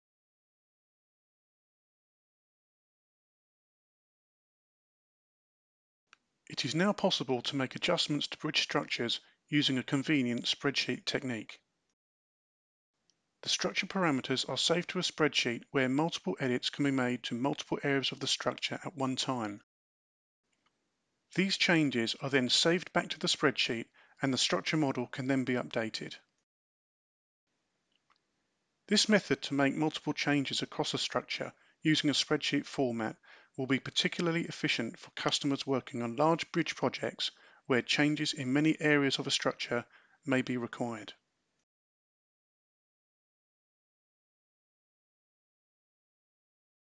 InfraWorks 2020 - Gestion d'un pont depuis un tableur Excel
Nouveauté InfraWorks 2020 - Export de le définition des composants d'un pont vers Excel et gestion de composants depuis le tableur (édition)